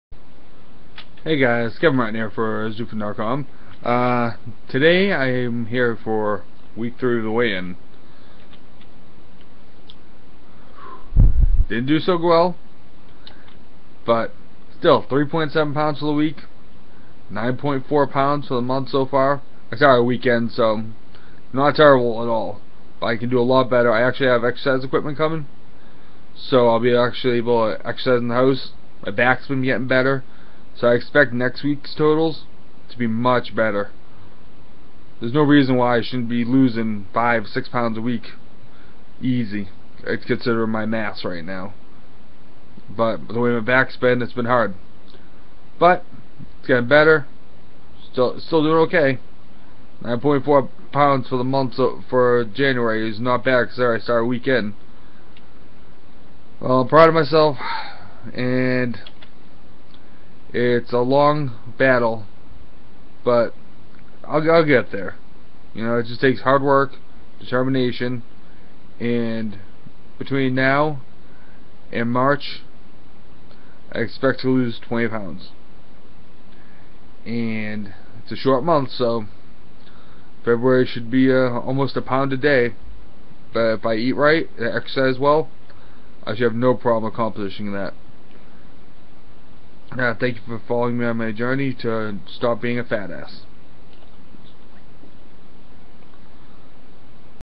Weight Loss Challenge Week 3
It was a slow but steady week. I made some progress towards my goal of losing 100lbs this year. I'm getting some workout equipment next week. So hopefully that will help me reach my goal faster.